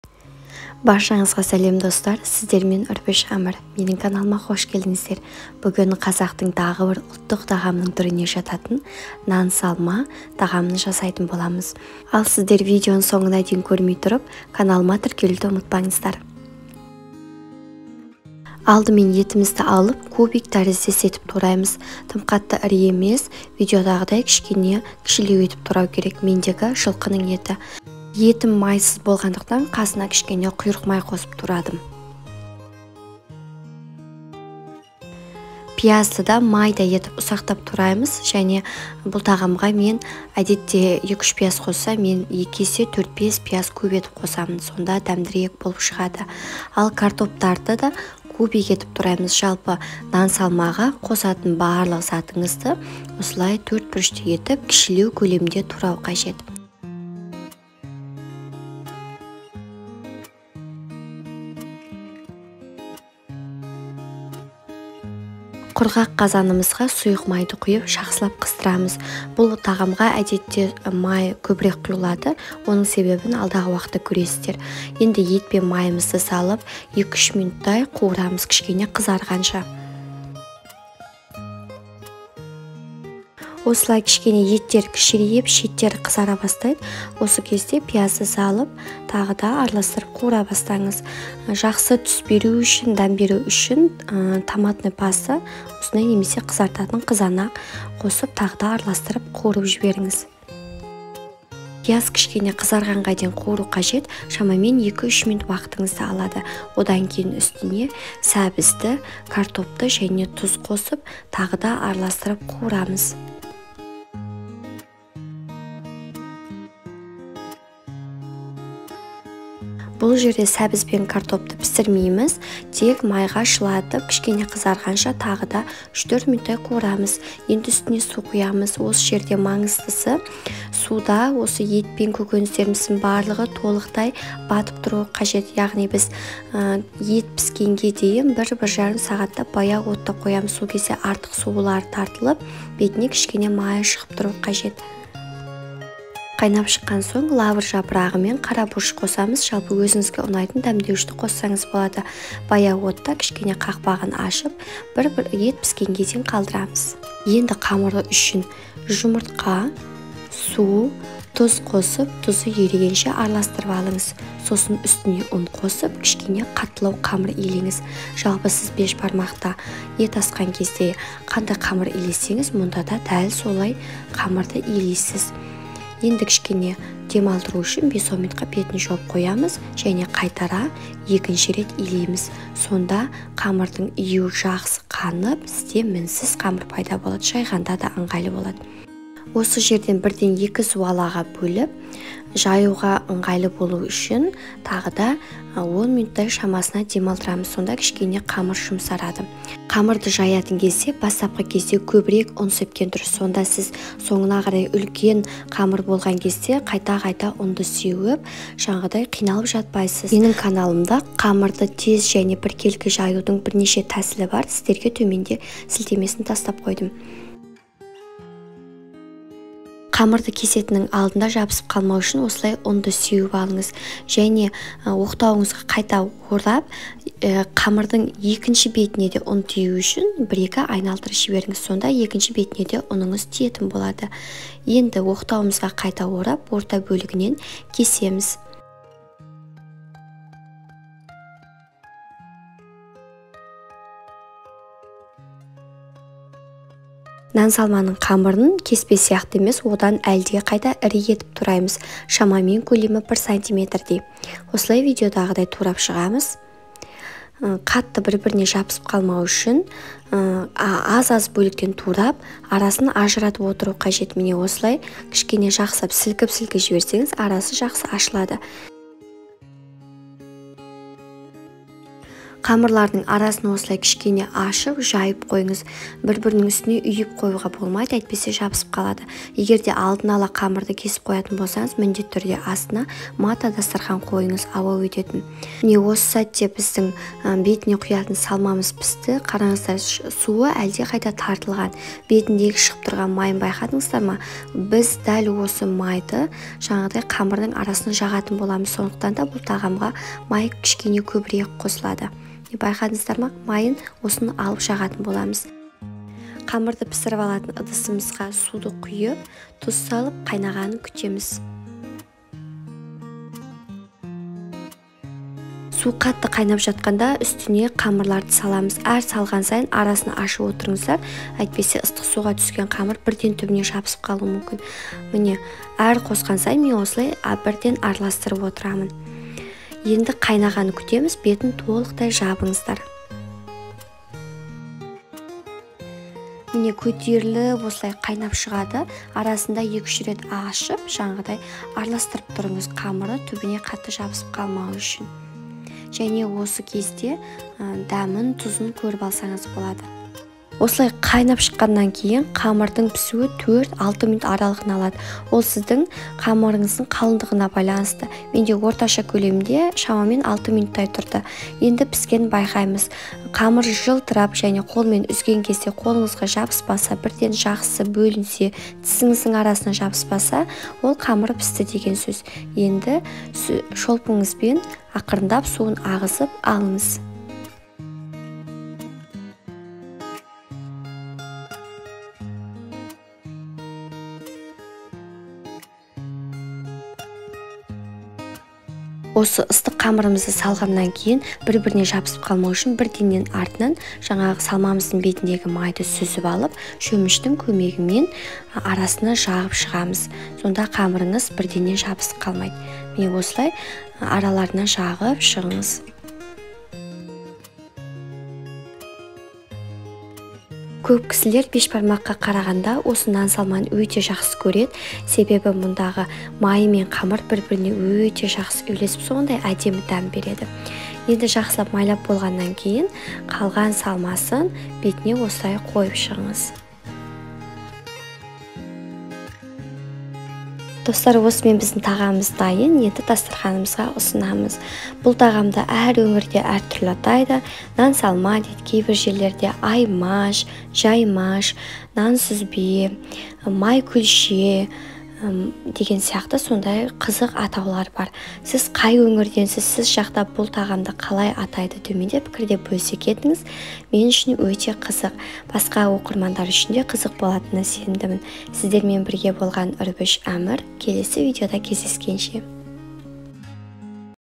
НАН САЛМА. Ең тойымды, дәмді ұлттық тағамымыз. Жаймаш, аймаш, сүзбе нан
ТІРКЕЛУ кнопкасының жанындағы ҚОҢЫРАУДЫ 🔔 басуды ұмытпаңыз, сонда келесі видеоны қалыс қалдырмайсыз. Сіздің менімен бірге болғаныңызға өте қуаныштымын

Құрамы:
Салмасына:
Ет - 300 гр
Құйрық май - 60/70 гр
Пияз - 4 шт
Картоп - 5 шт
Сәбіз - шт
Қуыруға май - 120 гр
Тұз, қара бұрыш, лавр жапырағы

Жаймасы үшін:
Жұмыртқа - 1 шт
Су - 150 мл (1 пиала)
Тұз - 1 шәй қасық толтырылған

Қайнап тұрған суда 5-6 мин піседі

Ингредиенты:
Для начинки:
Мясо - 300 гр
Курдючный жир - 60/70 гр.
Морковь - шт.
Масло для жарки - 120 г
Соль, черный перец, лавровый лист

Для тесто:
Яйца - 1 шт.
Вода - 150 мл (1 стакан)
С солью - 1 ч. л.

Варить в кипящей воде 5-6 минут